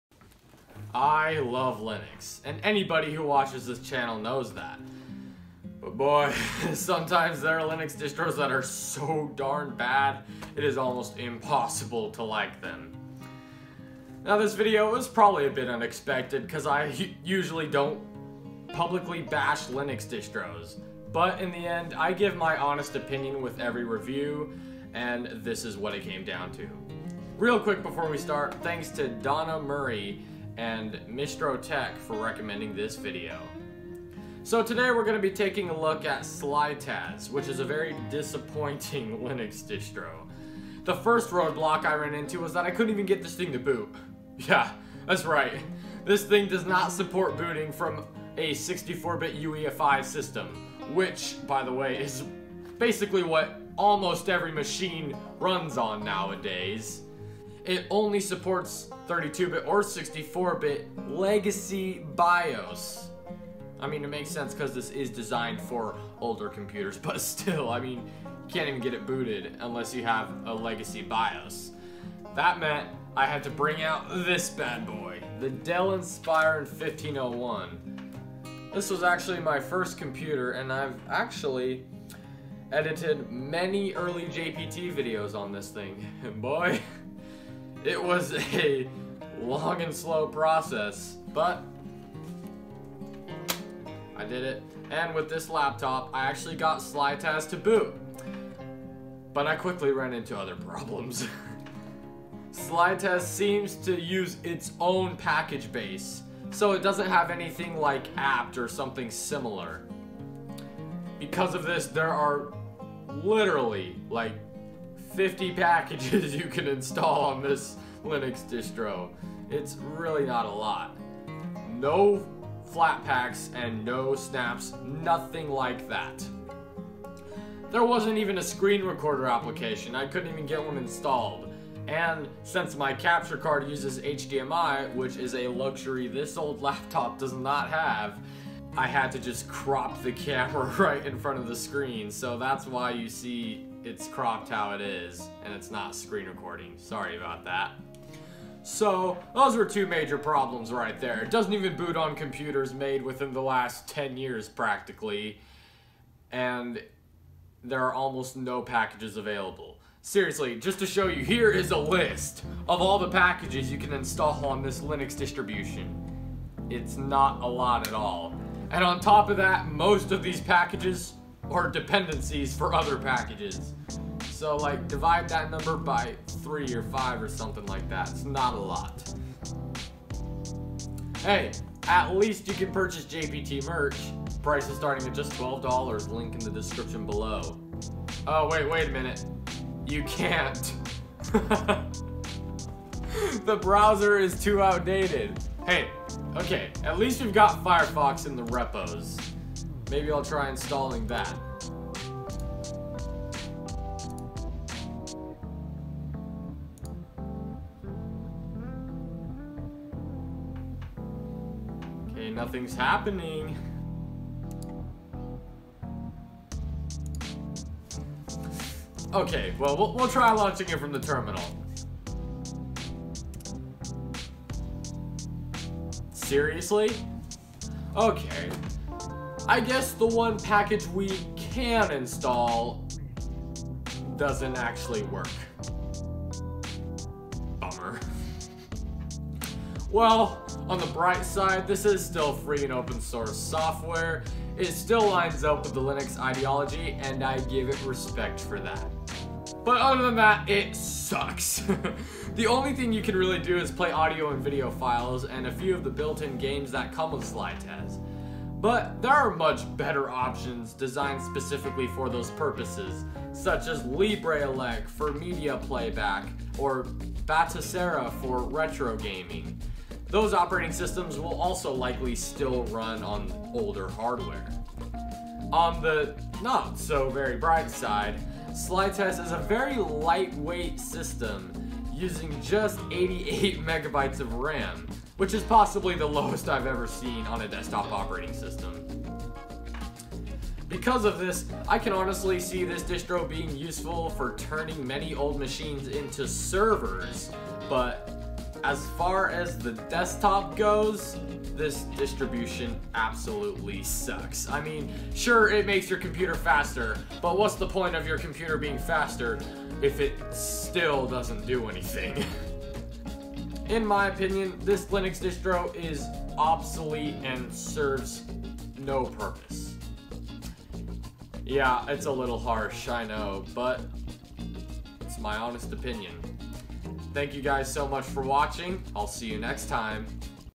This Linux Distro is AWFUL
If you have a Raspberry Pi 4 / 400, download and install Shock OS today